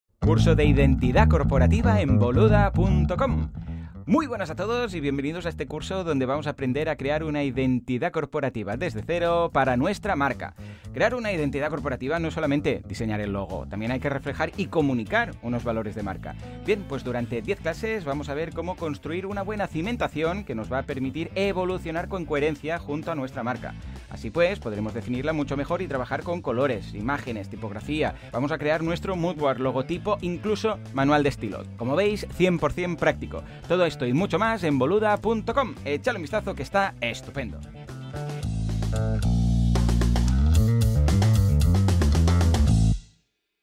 Curso de identidad corporativa
En este curso aprenderemos a crear una identidad corporativa lean desde cero para nuestra marca, para reflejar y comunicar nuestra imagen.